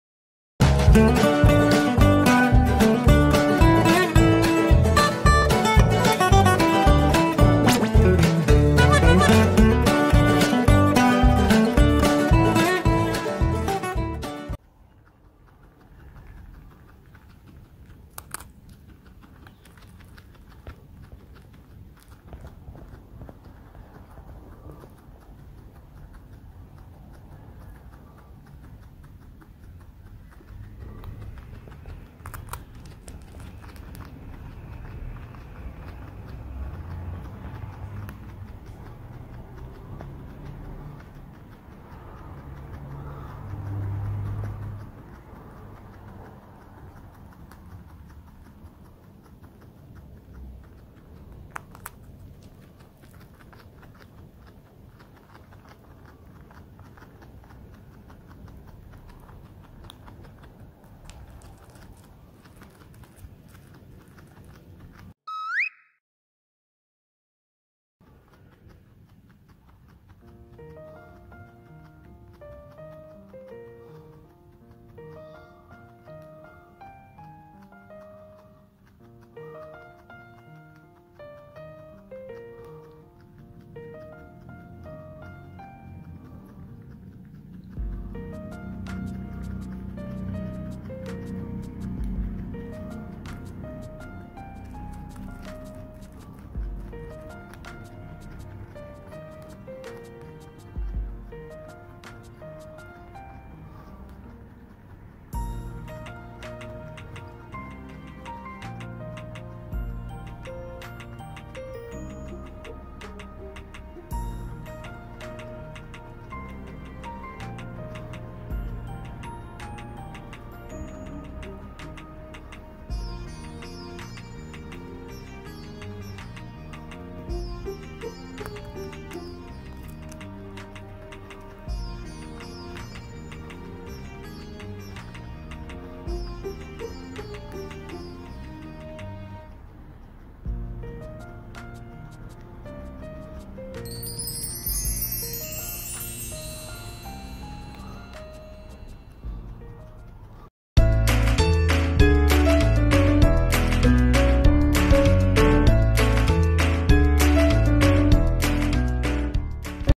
くるみちゃんがそばに居てくれるだけで幸せ。毎日ありがとう。
名    前 : くるみ
性    別 : おとこのこ
誕生日 : 2019年4月17日(推定)
平成産まれの元気な男の子のくるみちゃんと、くるみちゃんがだ～いすきなＯＬあなちゃんの日常動画です👧🐹(たまに👦も登場しますw)

【内容紹介】
くるみちゃんがもぐもぐ食べてる姿を見るだけで幸せ。
今日も元気に生きててくれてありがとう😊❤❤